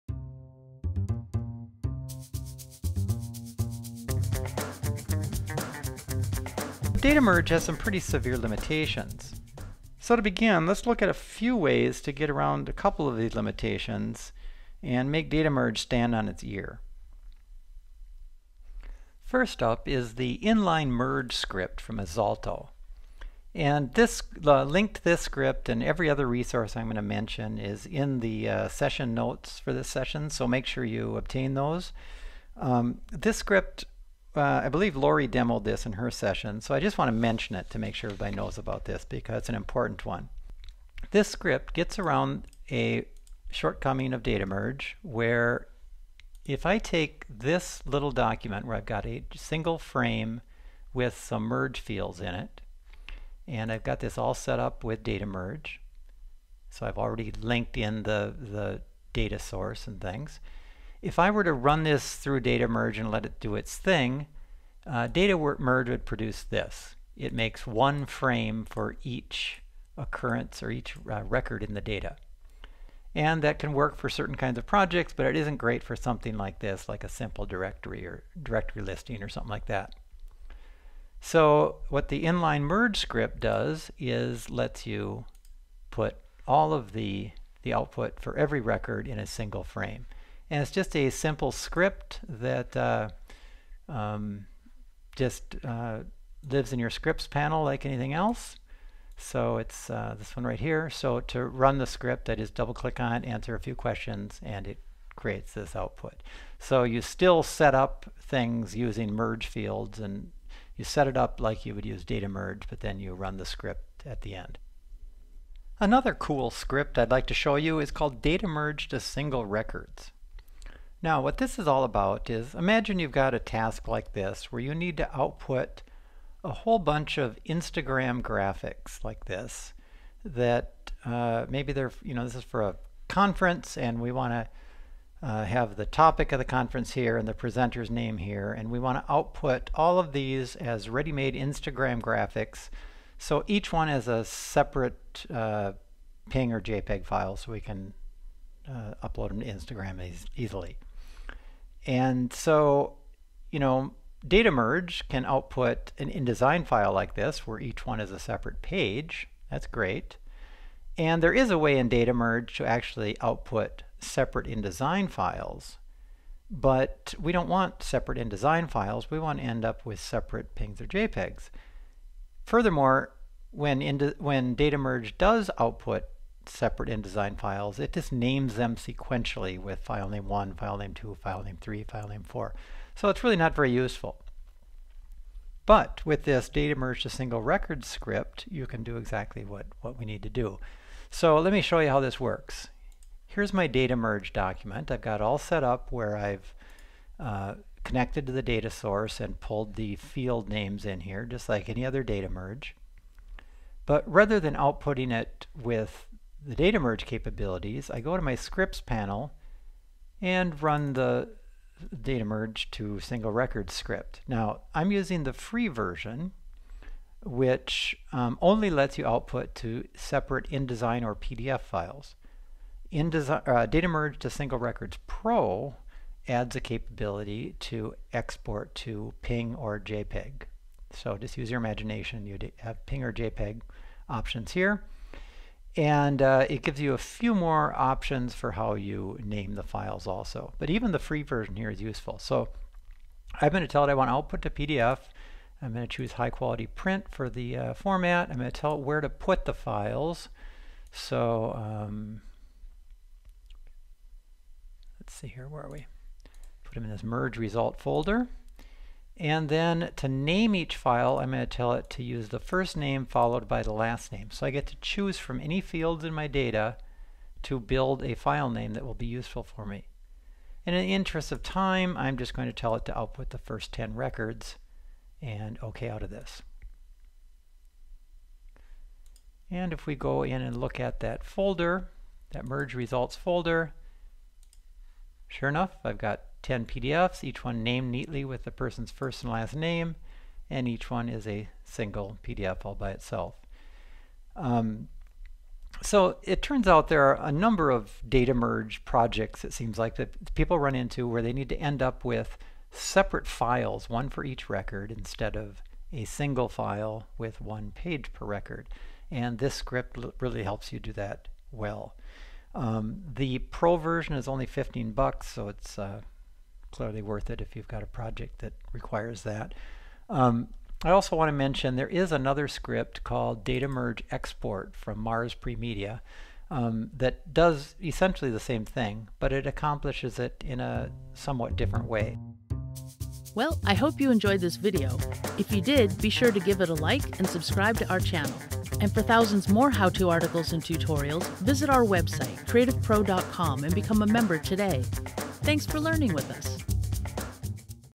InDesign: How to Improve Data Merge with Scripts (Video Tutorial)
In this InDesign video, Keith Gilbert shows off a couple of third-party scripts to help expand the built-in capabilities of Data Merge. From being able to merge into one continuous listing to exporting directly to individual graphics files, these scripts take InDesign’s data merge beyond the basics.

This tutorial was taken from Keith’s “Beyond Data Merge: Tools for Automation in InDesign” session at CreativePro’s Design + Data Summit.